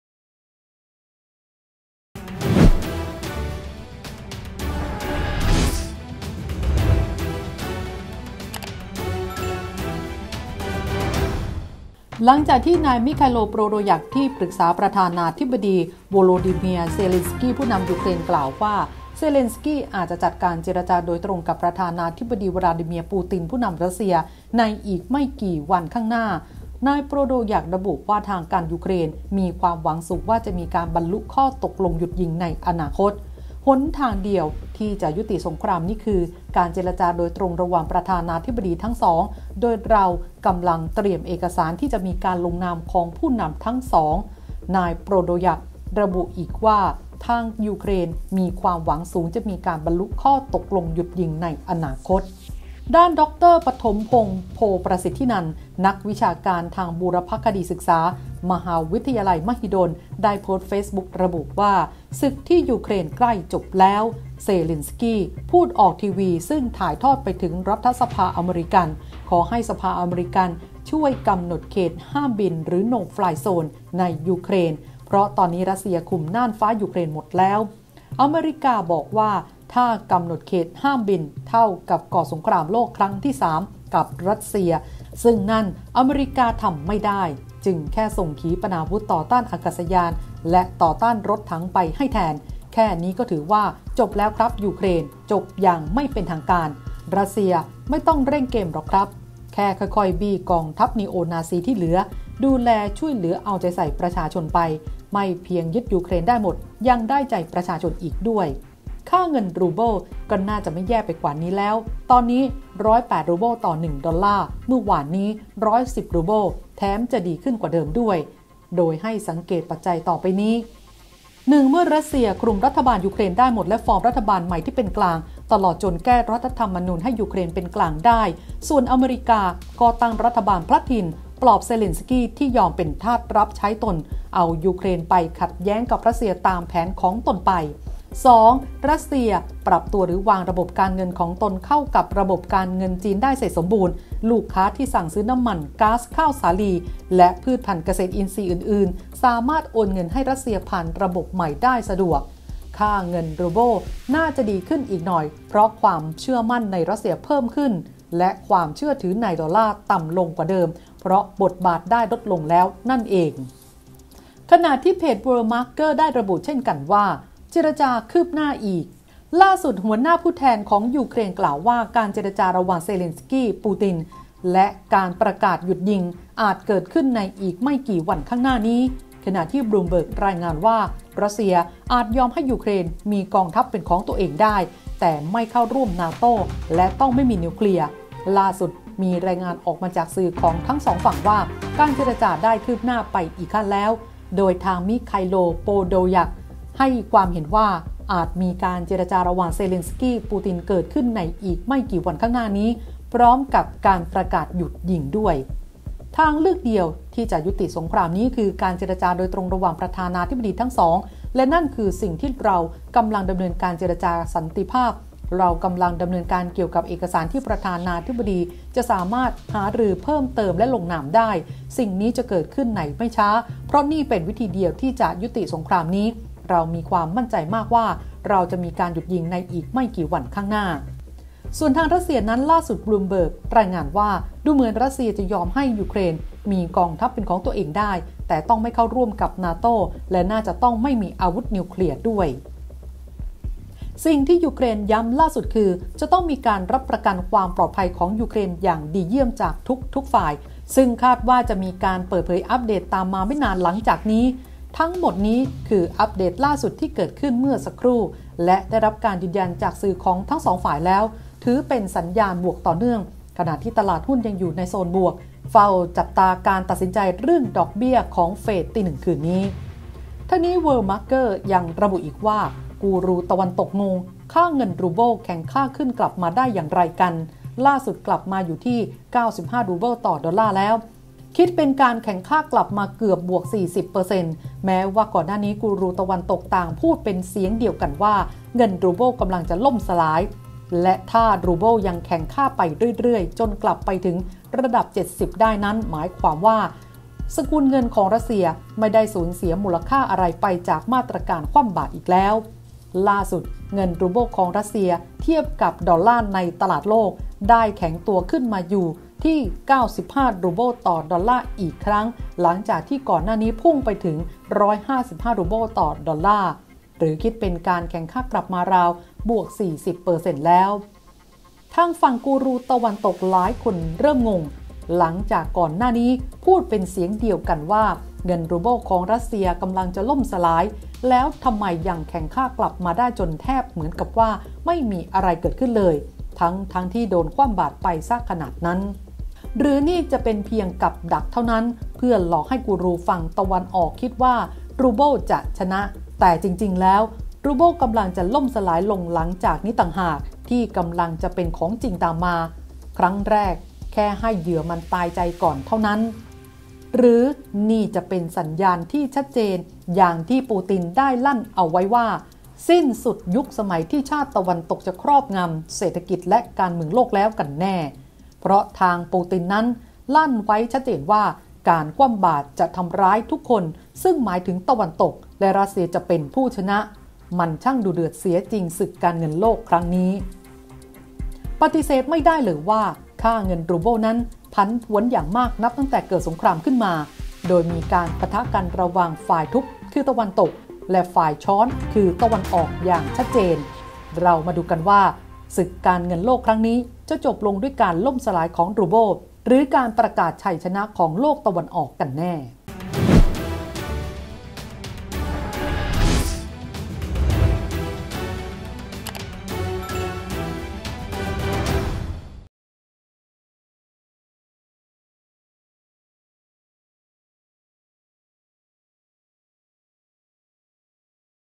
ศึกยูเครน ใกล้ปิดฉาก! "นักวิชาการ" เผย สงครามจบอย่างไม่เป็นทางการ| ข่าวเด่น | TOP NEWS
"ดร.ปฐมพงษ์"  ศึกรัสเซีย-ยูเครน ใกล้ปิดฉากอย่างไม่เป็นทางการ  คาด อาจมีการเจรจาระหว่าง"ปูติน-เซเลนสกี"ไม่กี่วันข้างหน้า พร้อมกับการประกาศหยุดยิง ส่วน ค่าเงินรูเบิล ดีดขึ้น ล่าสุด 110 รูเบิลต่อ 1 ดอลลาร์
โฆษณาติดต่อ